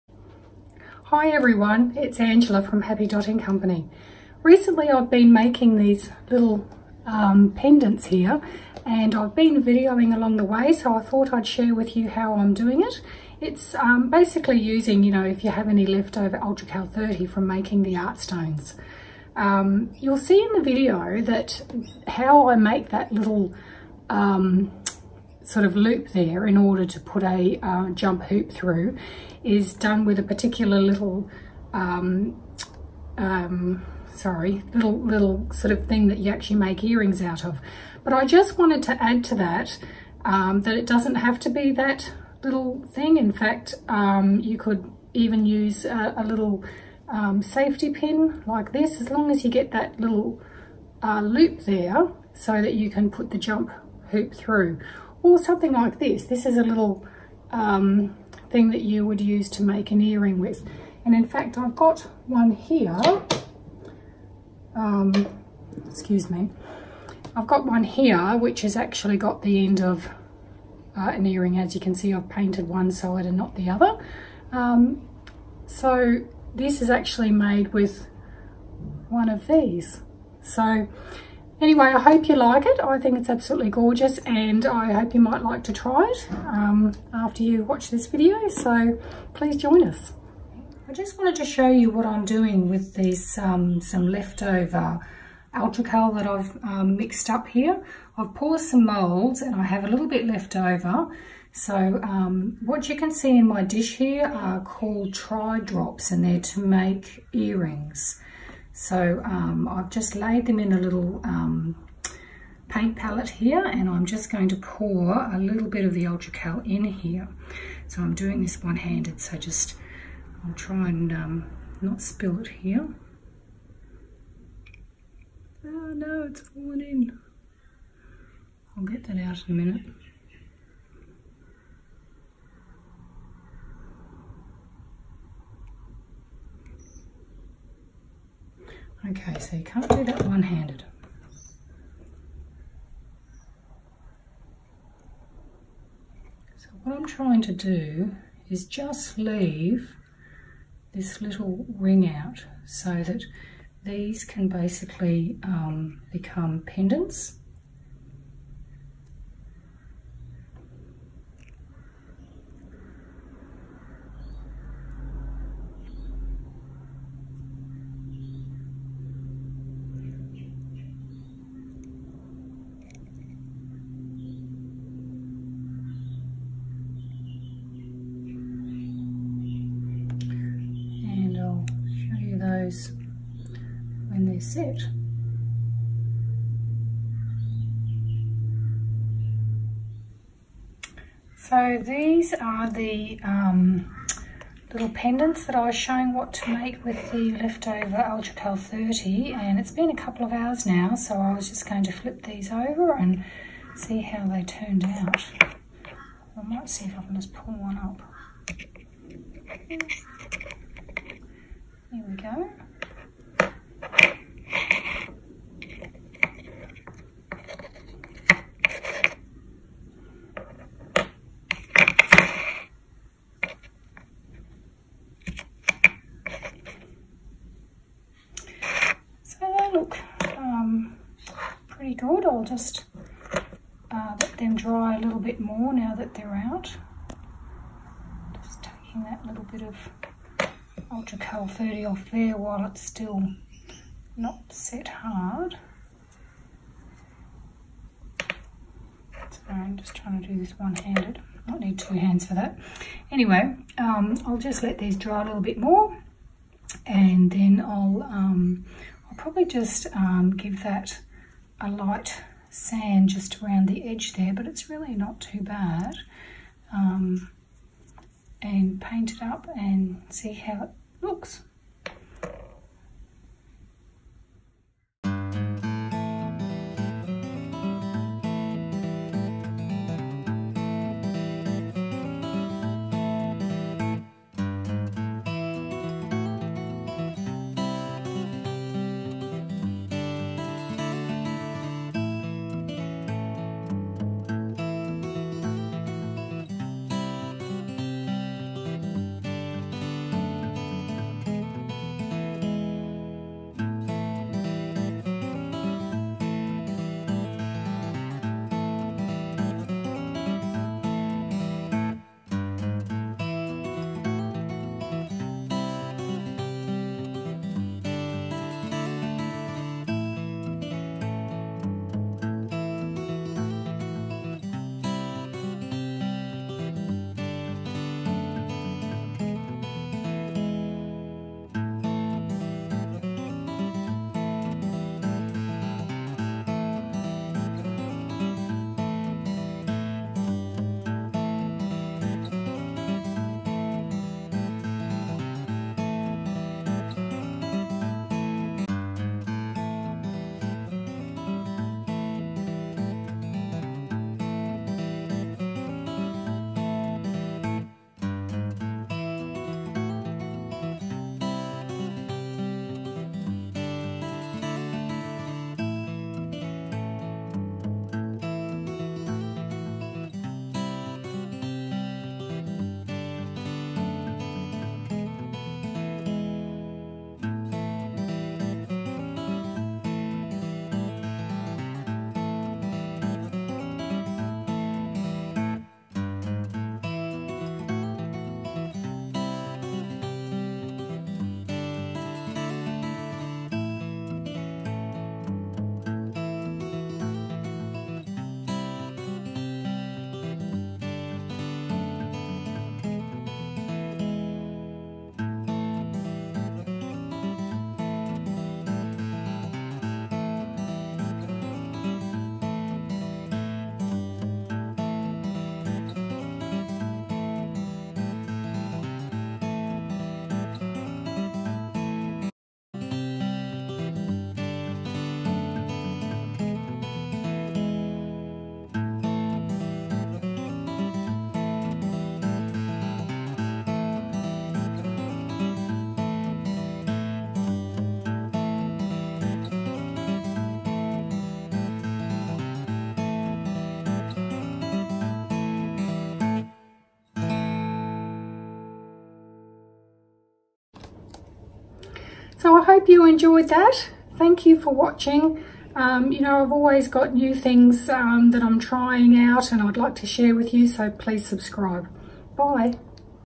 How to make Pendants from your leftover Art Stone mixture
Many people are already enjoying making their own Art Stones with Happy Dotting Company molds and sometimes have leftover mixture - here's an option of how to use it and create a beautiful pendant.
We ship to most places in the world via our Etsy Shop.

(conditions apply)

(conditions apply)